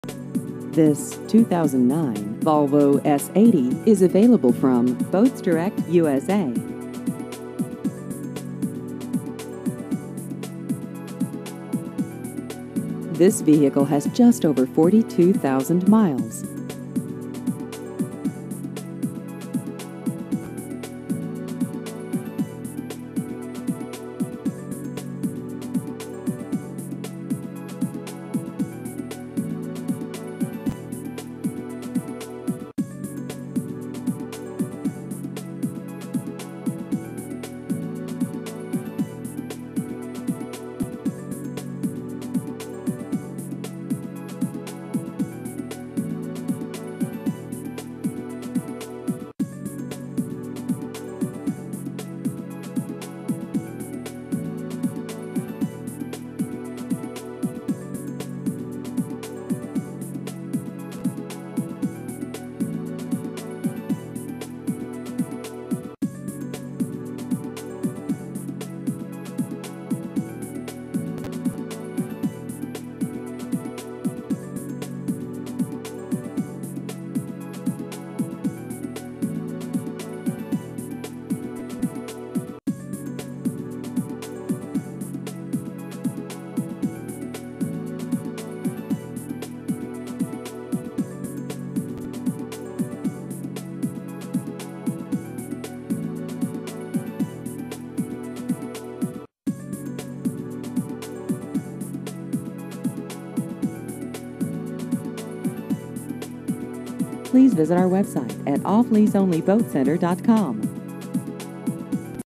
2009 Volvo S80 Used Cars Lake Worth FL
This 2009 Volvo S80 is available from Boats Direct USA.